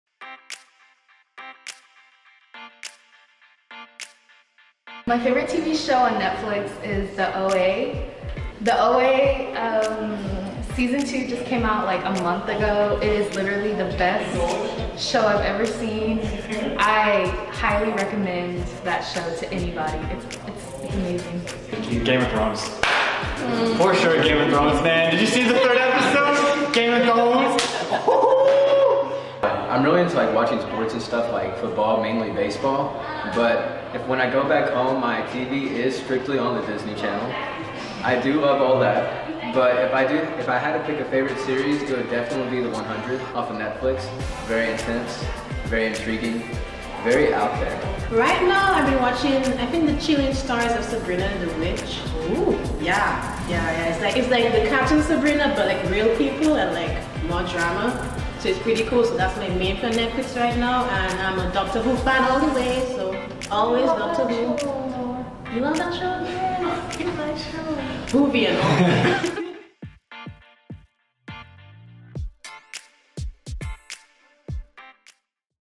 Learn about your Orientation Leaders favorite shows!
Meet Amelia, Trevor, DanLee, and Marissa, students at MSU Texas. They are only a handful of the new Orientation Leaders you'll see at Orientation this Summer. Learn about some of their favorite shows that appear in their Netflix queue and on television.